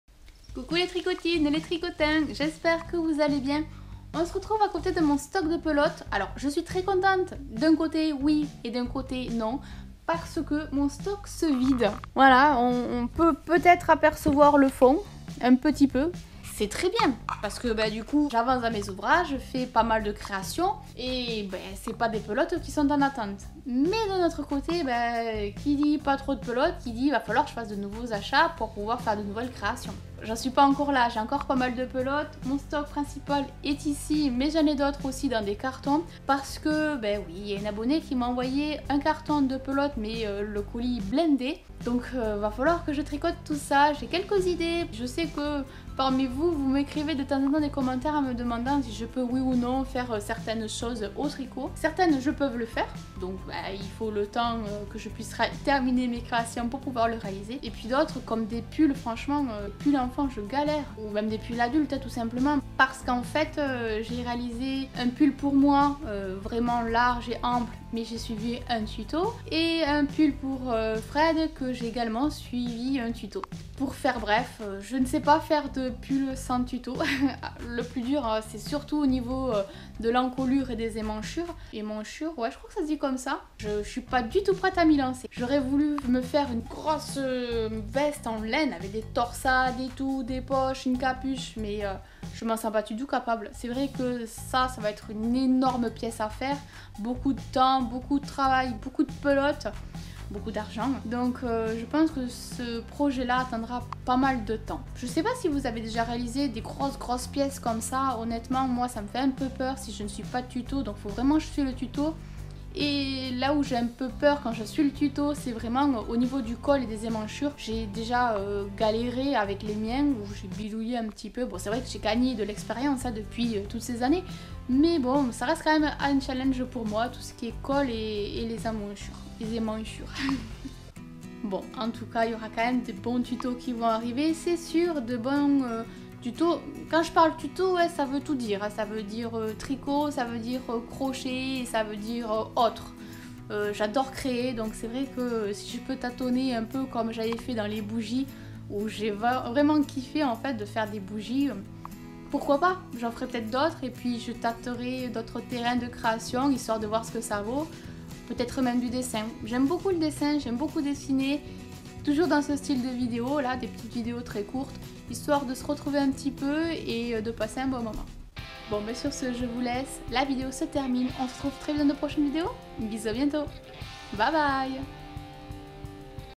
Les grosses créations me font peur ! 😰
Coucou ! on se retrouve dans une petite vidéo où je vous présente ma petite réserve de pelote et des grosses pièces que l'on me demande en tuto.
Et vous avez vous du stock? ^^

Mon Snapchat: lestutosdecely

Si tu lis ce message, écris "tricot addict" ;)